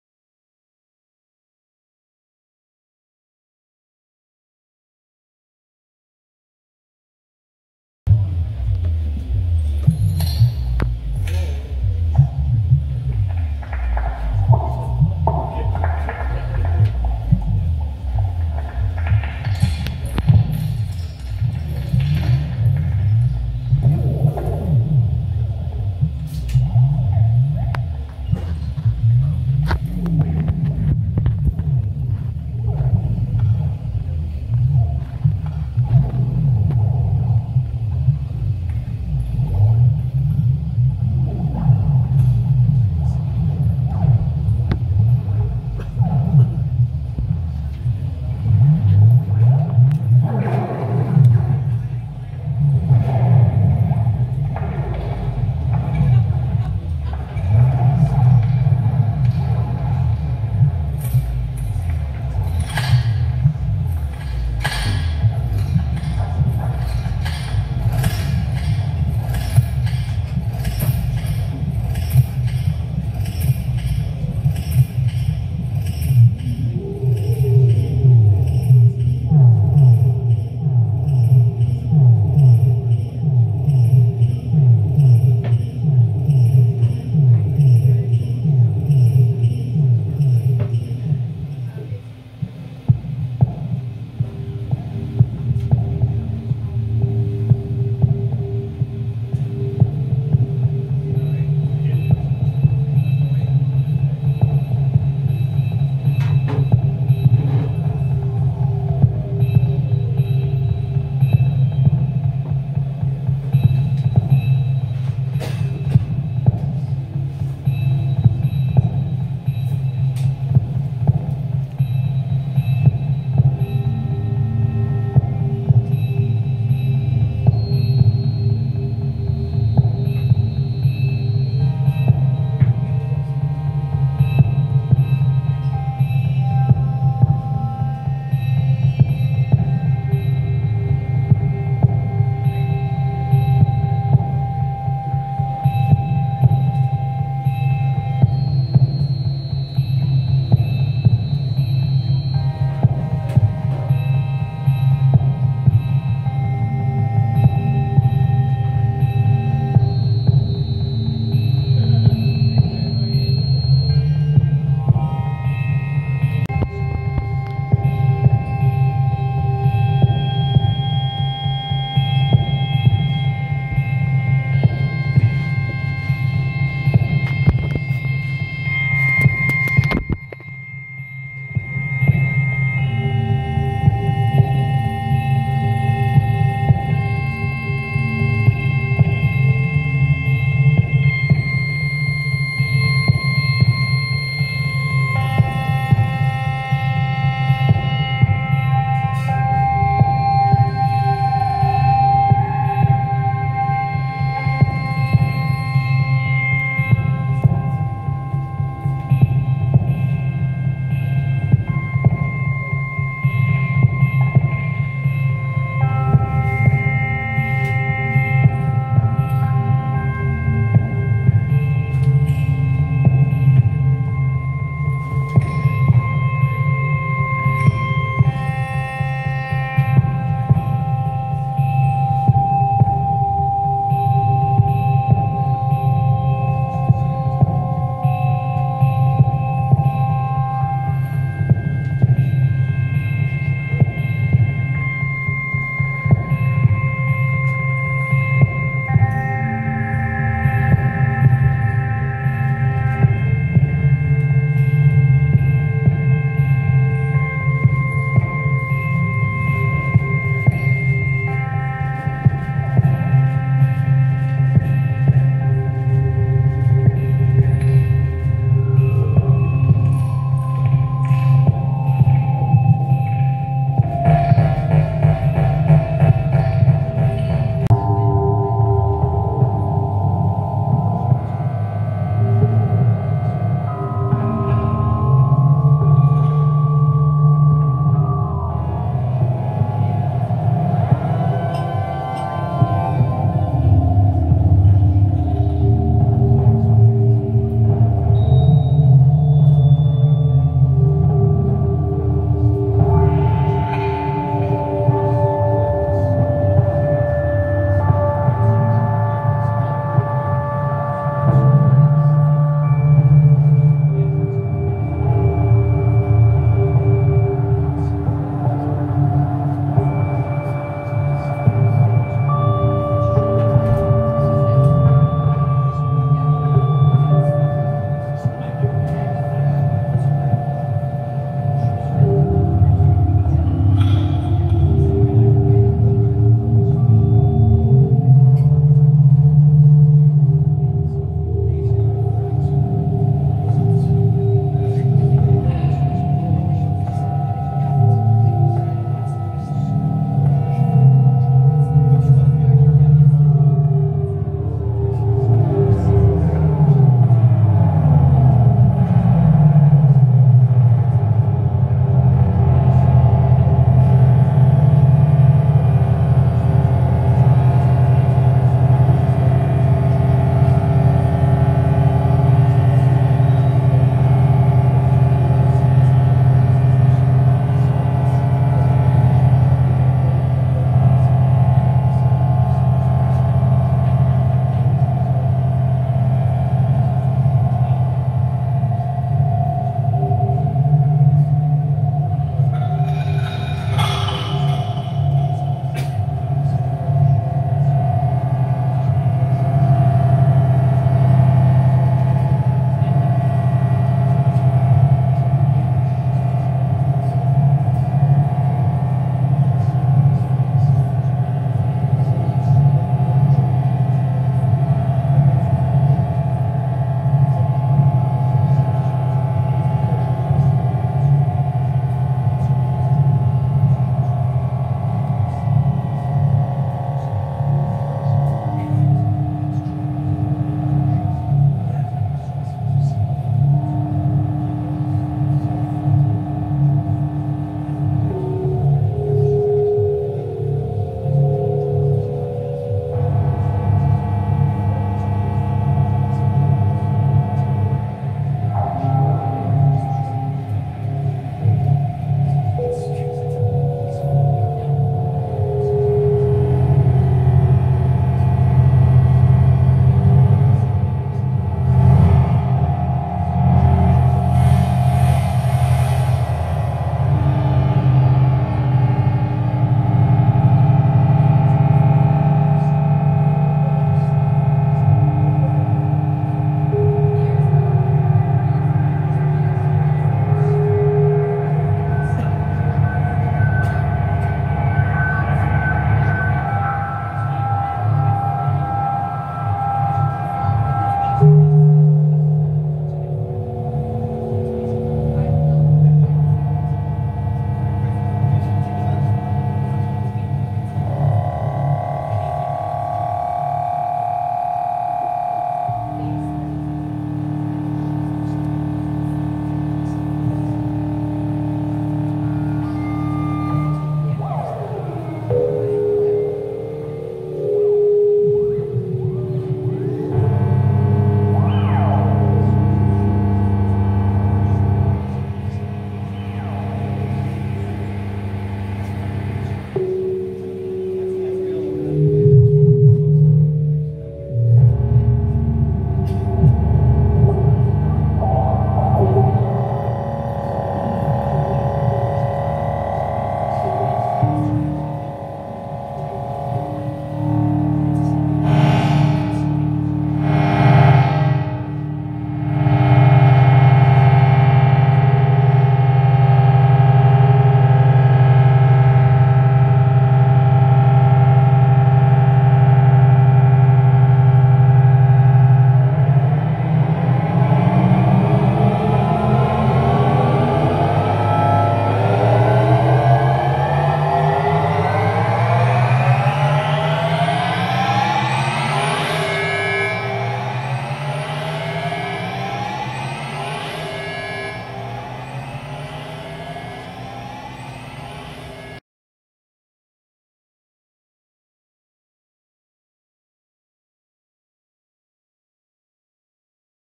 Stuart Clark at Handlebar, presented by Ambient Ping
Stuart Clark mesmerized the Ambient Ping crowd with his Euro Rack synth wizardry.